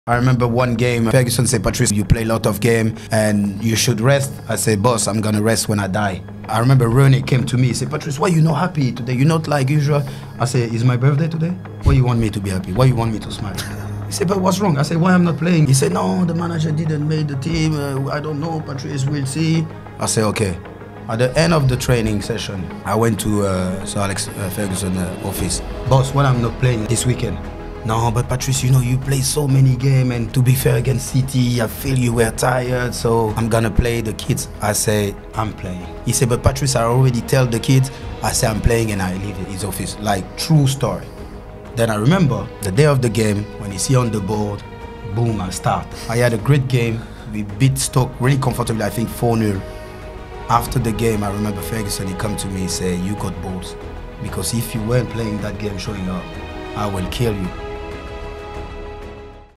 Patrice Evra About Confronting Sir Alex Ferguson For Not Playing
Speaker: Patrice Evra

just enjoy and share.

Source:
How Sir Alex Ferguson & David Gill Stopped Evra Going To Liverpool | The United Way Of Training